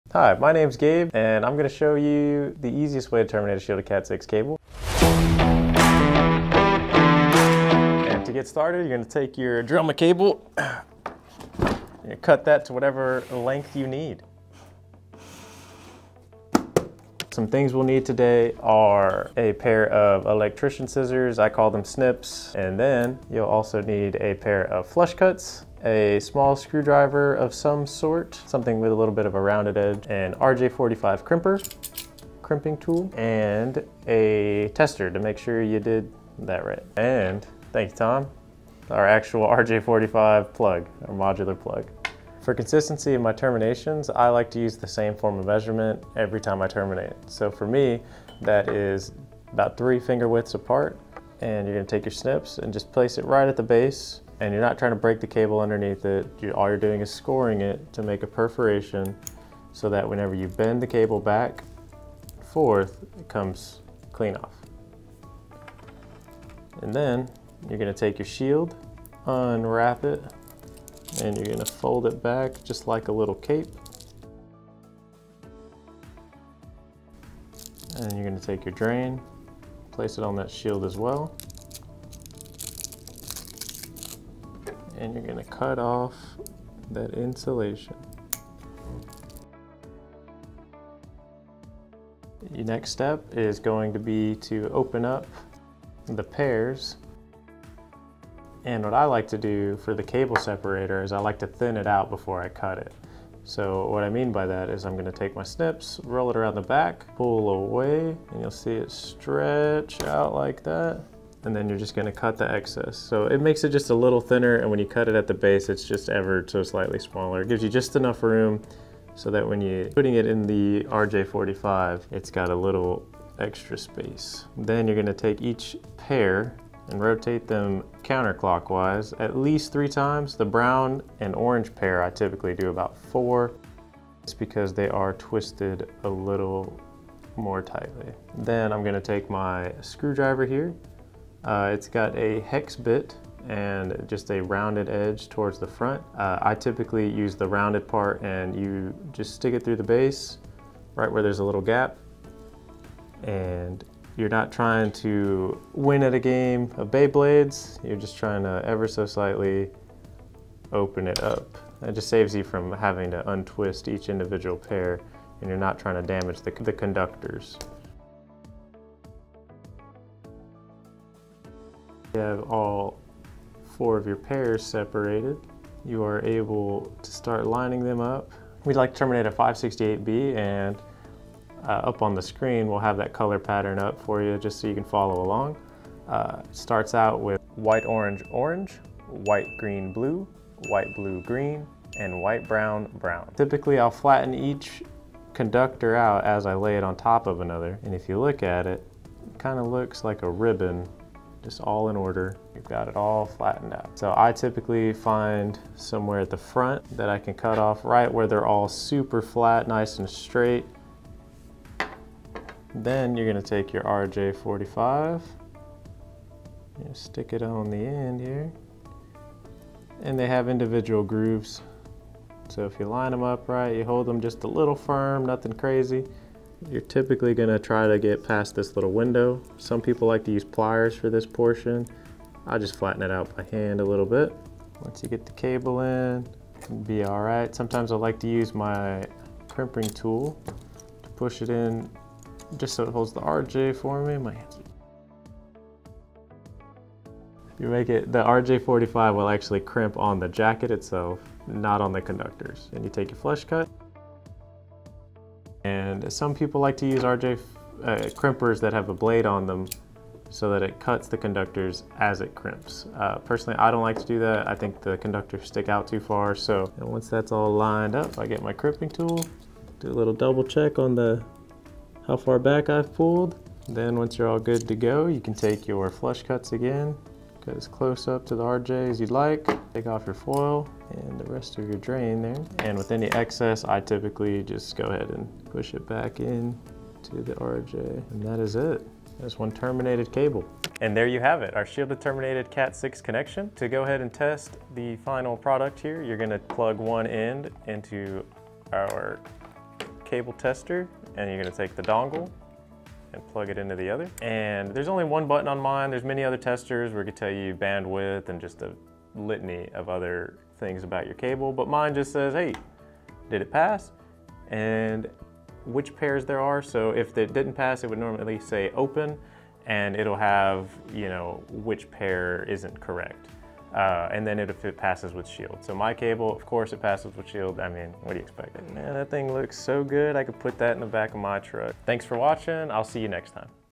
Simple & Fast Guide: Terminating Cat6 Cable with RJ45 Pass-Through Connectors!
🔌 DIY Cat6 Ethernet Cable Creation: Master the Art of Patching! 🔌

In the modern digital era, maintaining a seamless internet connection is paramount. Often, a tailor-made Ethernet cable is the key to flawless connectivity. Dive into our comprehensive guide as we detail every step in crafting a professional-grade Shielded Cat6 Patch Ethernet Cable.

🕒 Timestamps:

00:00 Introduction
00:09 Kick-off
00:18 Required Tools & Materials
00:45 Cable Preparation
01:37 Separating Cable Pairs
03:00 Arranging by Color Codes
03:33 Inserting into RJ45
04:25 RJ45 Crimping Process
04:55 Cat6 Cable Verification

Benefit from our lucid instructions coupled with seasoned advice, and handcraft a Shielded Cat6 RJ45 Ethernet cable tailored to your requirements. Say farewell to settling for generic cables that don’t resonate with your networking demands in terms of length or grade.

Whether you're a tech aficionado, a DIY enthusiast, or keen on enhancing your home network, our tutorial is your roadmap to constructing superior Ethernet cables.

Elevate your connectivity experience; embrace the convenience of a custom-made Ethernet solution ideal for your residence or workspace.